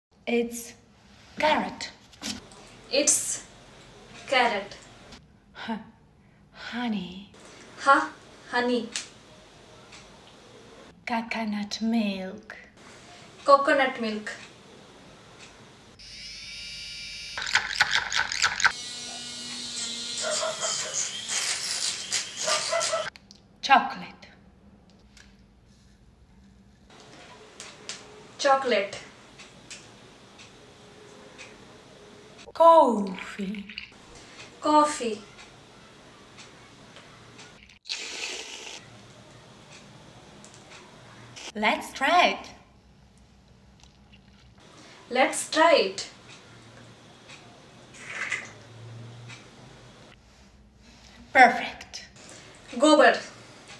Corrot Latte | Perfect | Part - 15 | @RainbowFunFoods
Hi Frends,.! In This Video We Will Be Doing The Carrot Latte. Do Comment Down Your Ideas For Our Next Challenge So That We Keep On Entertaining You All.